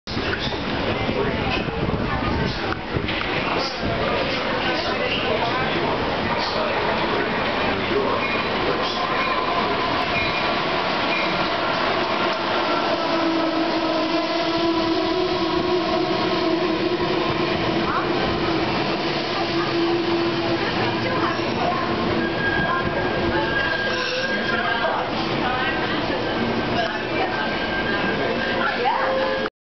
Amtrak Keystone #650
Arriving at Trenton Station on Track 1 at 2:33 p.m. to pick up & drop off passengers before heading to Newark Penn Station & New York with ASEA AEM7 #953 leading & Cab Control Car #9649 bringing up the rear. I gave the engineer a friendly wave.